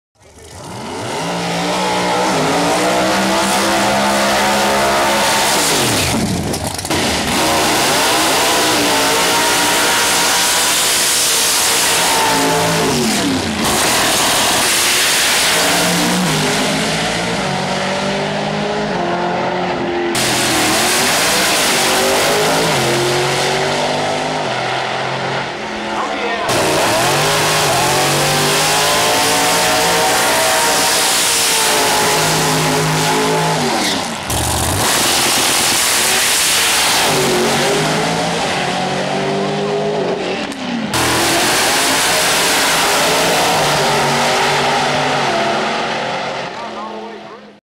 Pro Modified Quick-8 Farmington Dragway 9/1/07
Footage of Pro Mod quick-8 from Farmington Dragway on 9/1/07.